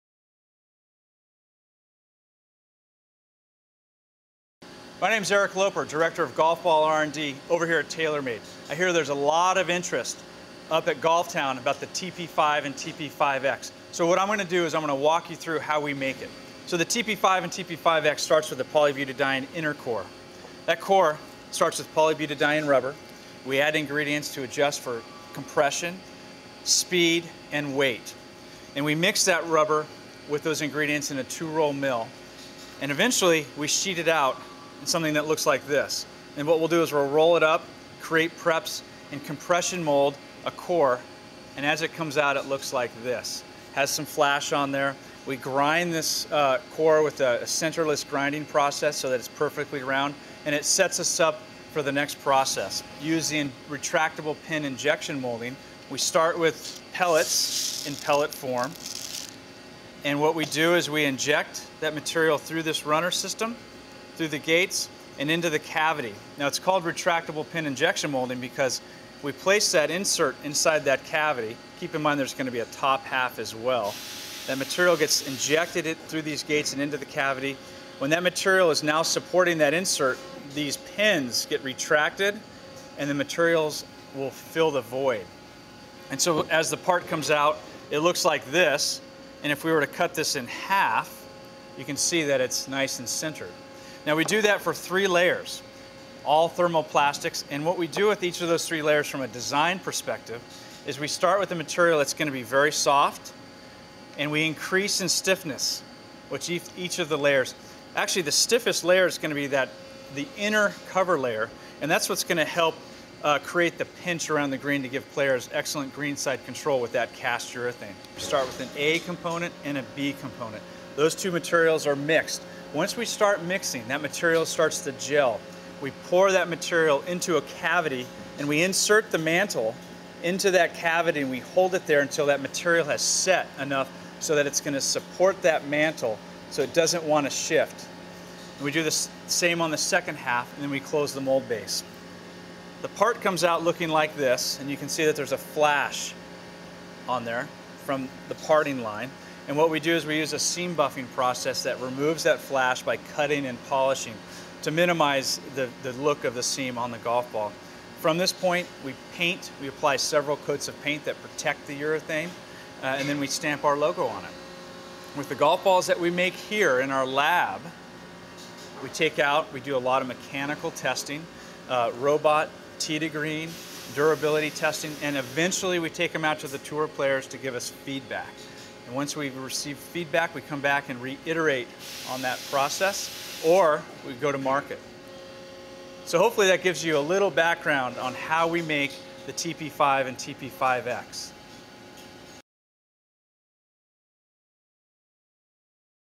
How It's Made: TaylorMade TP5 / TP5X Golf Ball
We caught up with Eric Loper, TaylorMade’s Director of Golf Ball Development and Research to find out how a TP5 golf ball is made.